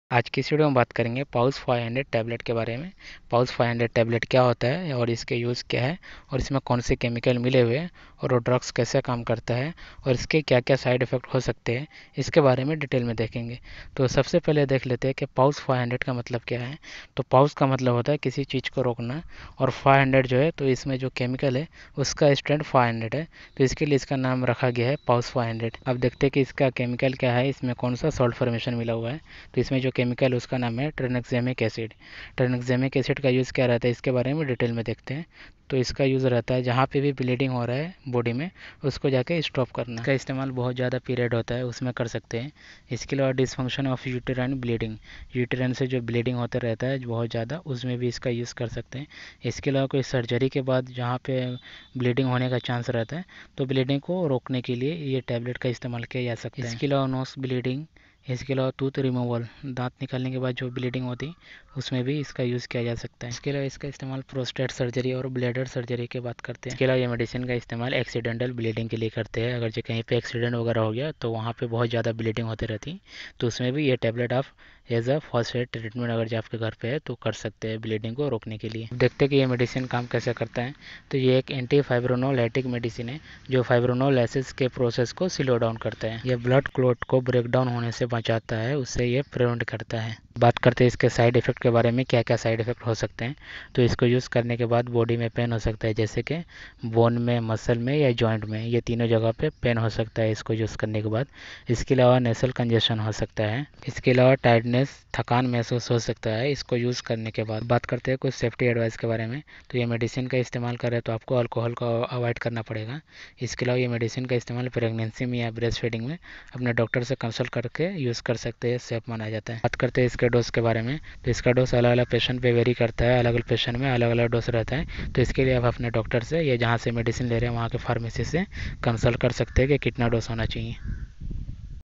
pause 500 tablet in hindi, uses side effects,how it's work medicine for heavy bleeding, heavy period
Pause 500 tablet review in Hindi

Pause 500 Tablet is an anti-fibrinolytic. It works by preventing the breakdown of clots which leads to stoppage of bleeding. This medicine must be taken in the dose and duration as advised by the doctor. You should not take it if you have any known allergy from this medicine.

The most common side effects include tiredness, nasal congestion, and pain in muscle, bone, or joint. Inform your doctor if you have undergone any cardiac surgery or you are suffering from any kidney disease.

This medicine is safe to use in pregnant or breastfeeding mothers. It is also safe to use in patients suffering from liver diseases. It is advised not to consume alcohol after taking this medicine as there can be excessive drowsiness.